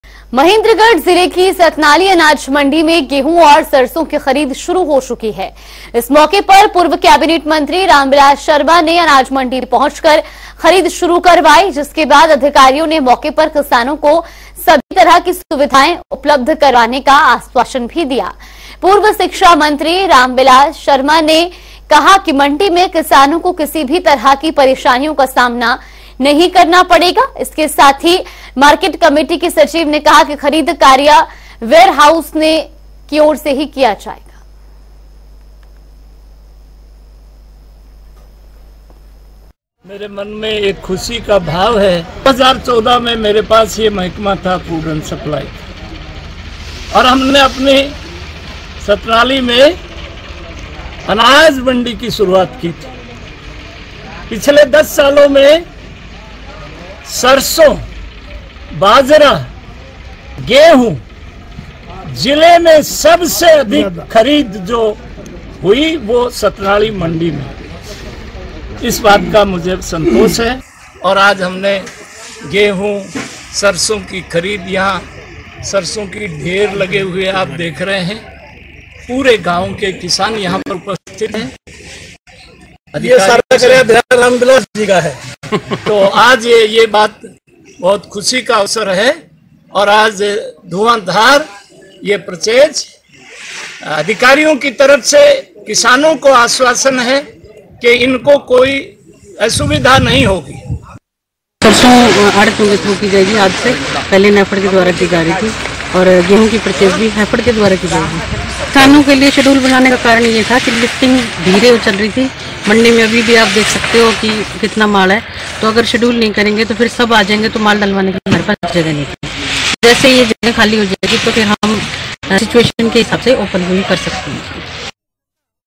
Ram Bilas Sharma ने किसानों को दिया आश्वासन, कहा- असुविधाओं का नहीं करना पड़ेगा सामना
Mahendergarh पहुंचे Ram Bilas Sharma, किसानों को दिया आश्वासन, कहा- असुविधाओं का नहीं करना पड़ेगा सामना